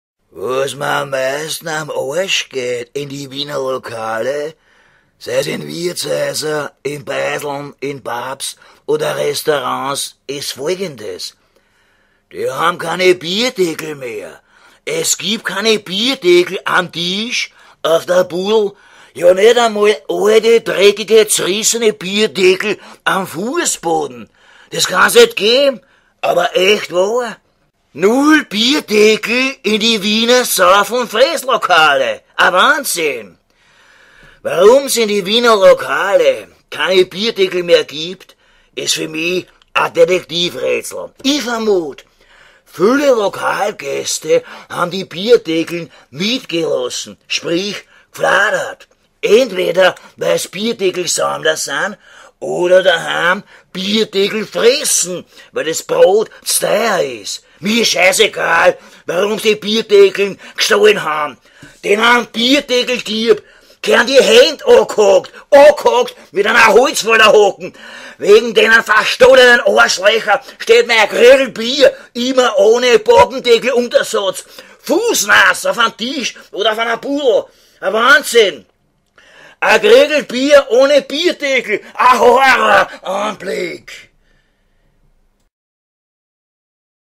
biertrinker schimpft
Der Wiener Bier-Sepp ärgert sich über Wiener Lokale, die keine Bierdeckel haben...  ein witziges Rumpelstilzchen.